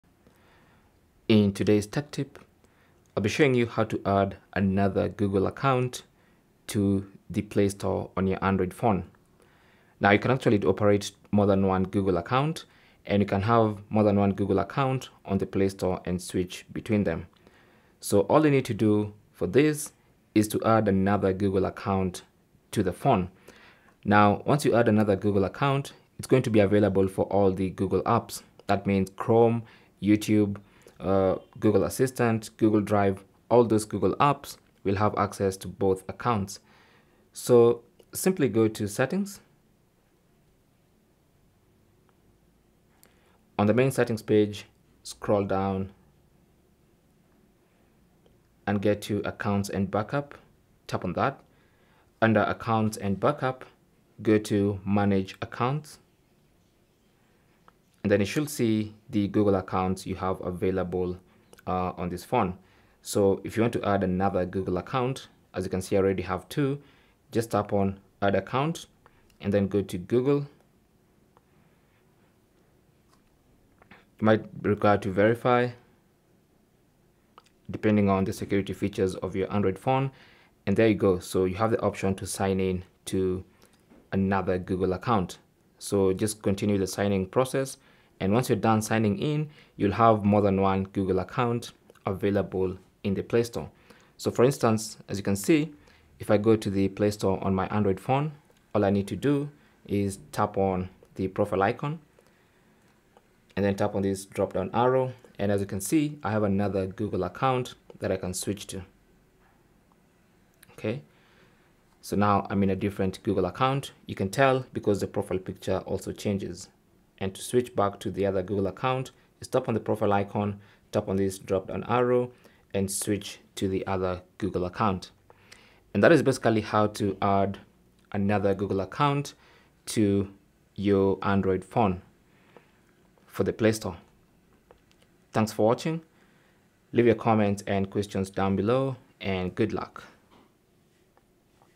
How To Add Another Google Account To Google Play Store
Learn how to add another Google account on Google Play Store.

Google Play Store is the hub for all Android users to download and manage their favorite apps, games, movies, and more. Having multiple Google accounts linked to your Play Store can offer various benefits, such as accessing different apps, sharing purchases, and maintaining separate app preferences.

Adding another Google account to your Google Play Store is a straightforward process that allows you to expand your digital ecosystem. In this tutorial video, we'll show you the steps to add another Google account to the Google Play Store.

First and foremost, you need to add a Google account on your Android device. Go to Settings, then scroll down and tap on Accounts & BackUp. From here, tap on Manage Accounts, then tap on Add Account.

Once the account has been added, go to the Google Play Store. Tap on your profile icon at the top right corner then tap on the drop-down arrow next to your account name. Find the account you'd like to switch to and tap on it.

How To Add Another Google/Gmail Account On Samsung Galaxy S23s

Galaxy S23 Ultra DUAL SIM 256GB 8GB Phantom Black:

Google Pixel Tablet:

Cell Phone Tripod Adapter